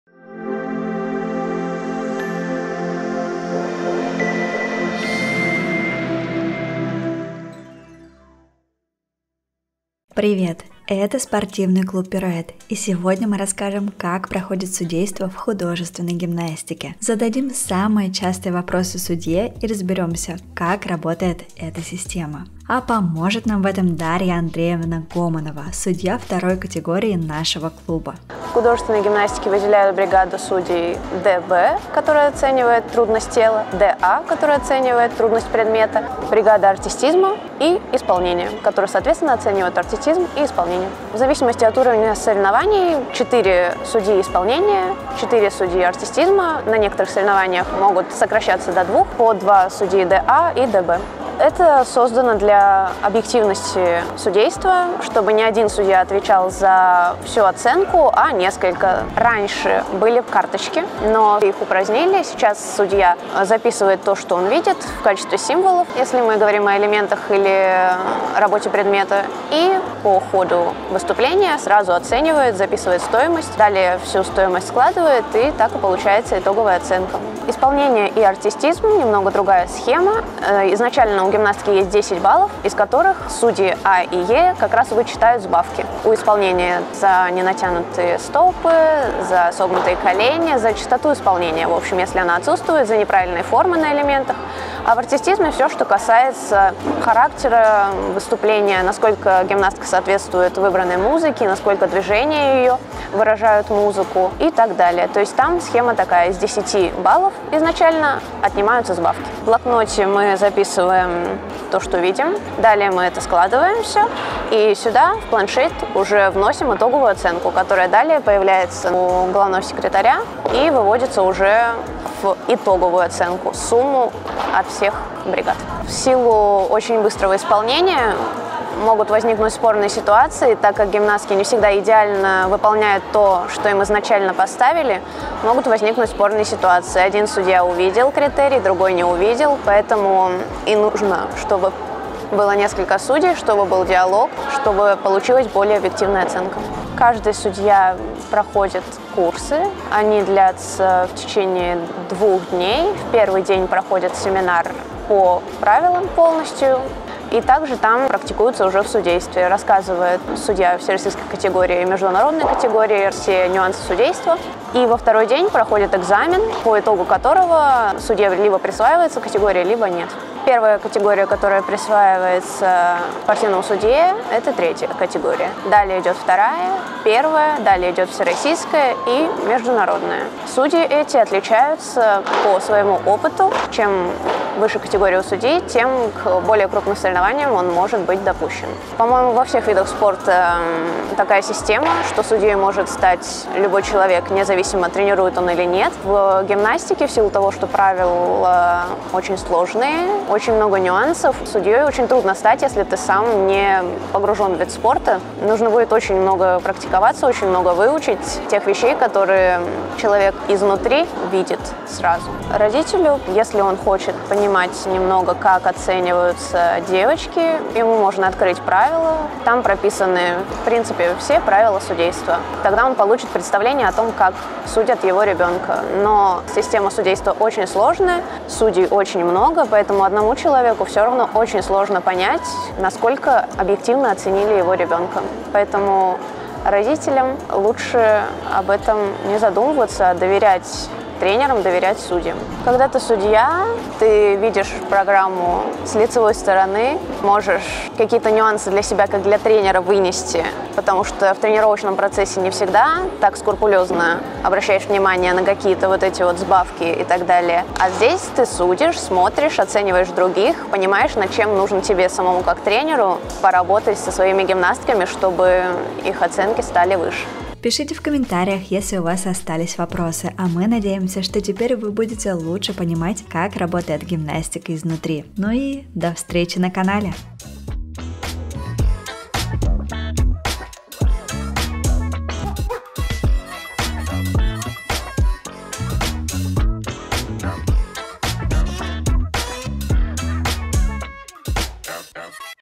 Судейство в художественной гимнастике: отвечаем на самые частые вопросы PIROUETTE
Спортивный клуб художественной гимнастики PIROUETTE